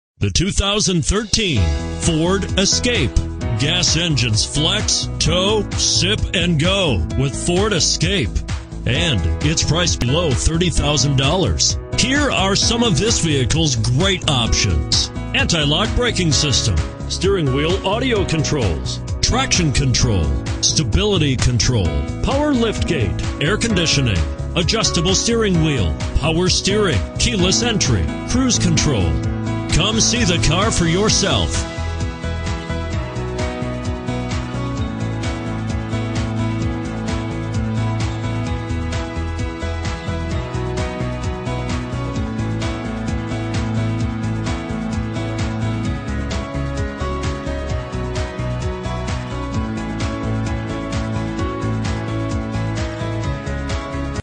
2013 FORD ESCAPE Old Bridge, NJ 130716
2013 FORD ESCAPE Old Bridge, NJ
Stock# 130716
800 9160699

Pick up this Sterling Grey Metallic 2013 Ford Escape, available today. Featuring Automatic transmission, it has 5 miles. This could be the one you've been searching for.